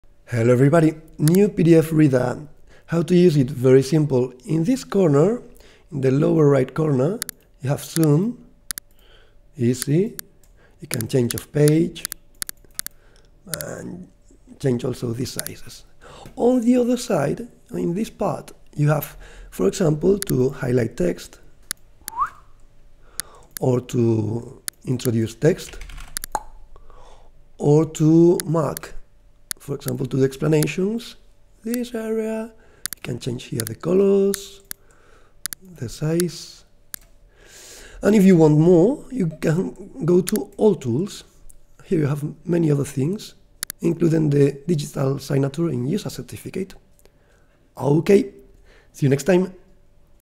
Draw, write, highligh, zoom... How To Use Adobe PDF Reader 2023 - Basic Guide For Beginners
Introduction to Adobe PDF Reader 2023 - Basic Guide For Beginners: Draw, write, highligh, zoom...